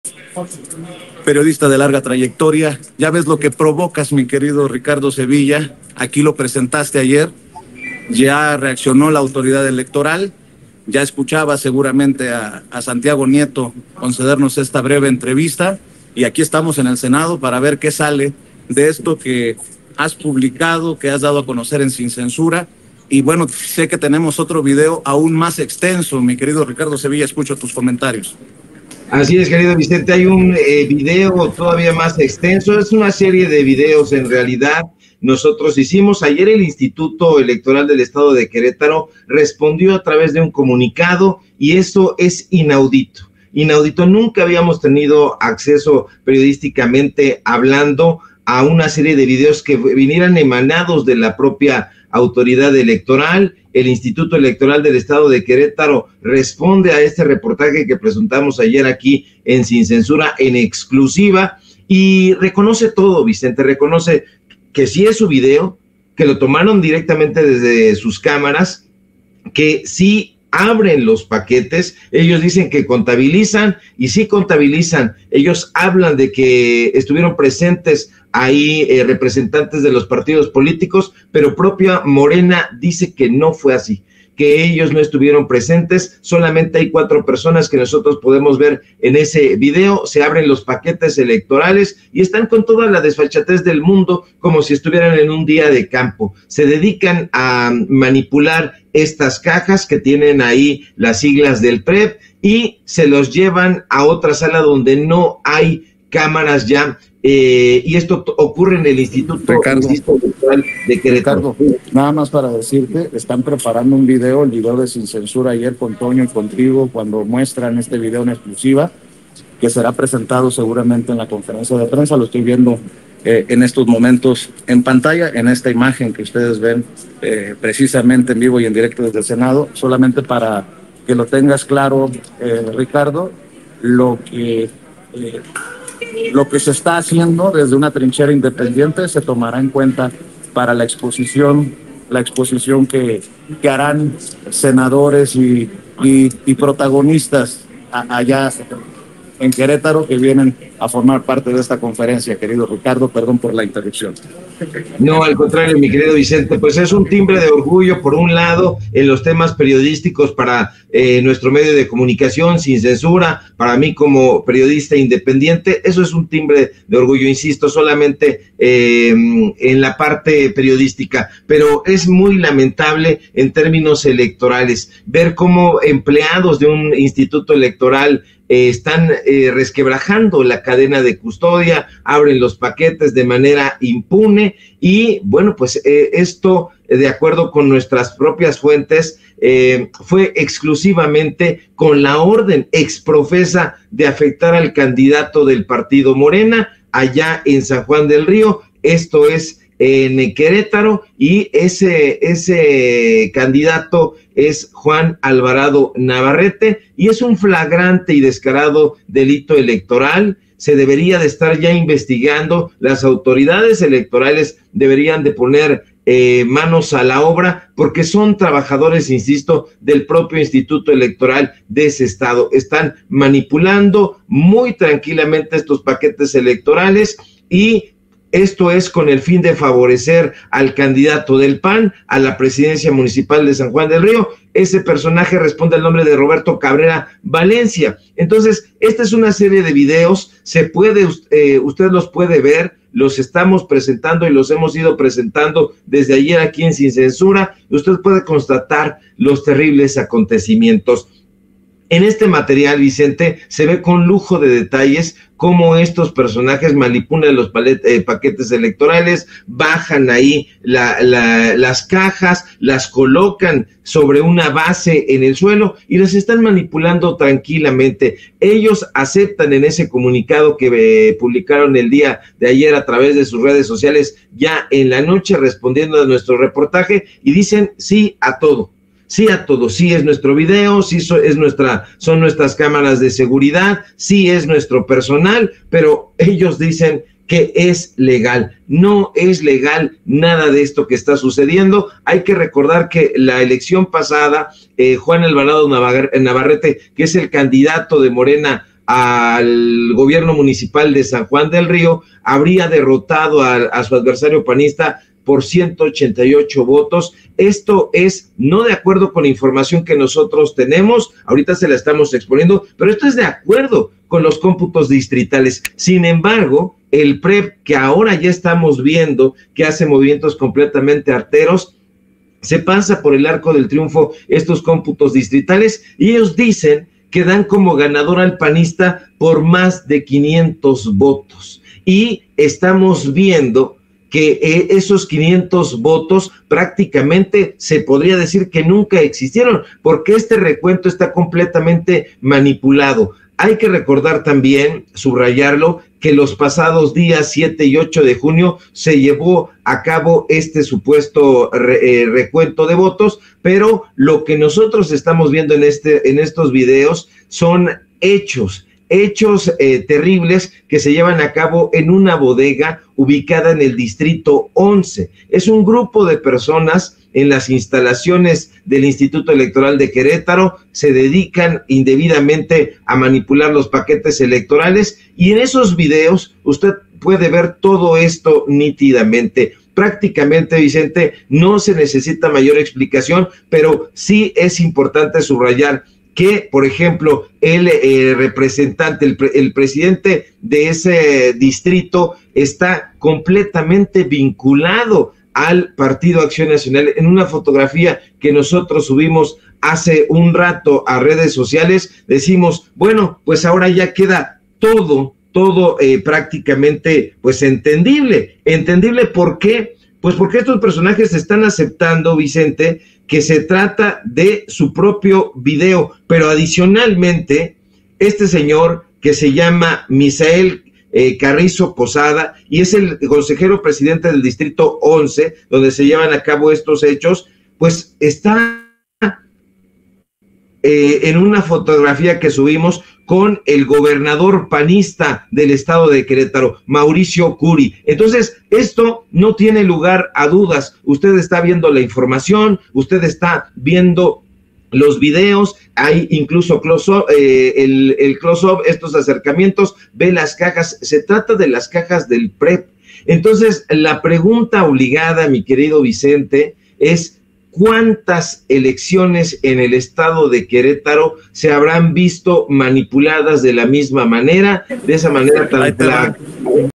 LAS AUTORIDADES ELECTORALES DEBERÍAN DE PONER MANOS A LA OBRA EN EL IEEQ, EXIGIÓ SEVILLA
El escritor y periodista Ricardo Sevilla señaló que el Instituto Electoral del Estado de Querétaro reconoció que los videos que presentó ayer en Sin Censura son verídicos. Sin embargo, aunque las autoridades electorales aseguran que estuvieron presentes representantes de partidos políticos, Morena señala que no fue así, porque ellos no participaron en esa actividad. Enfatizó que es lamentable que empleados del Instituto Electoral v10l4r4n la cadena de custodia para afectar al candidato del partido guinda, Juan Alvarado Navarrete. Enfatizó que las autoridades electorales Federales deberían tomar acción cuanto antes contra esta indebida manipulación de los paquetes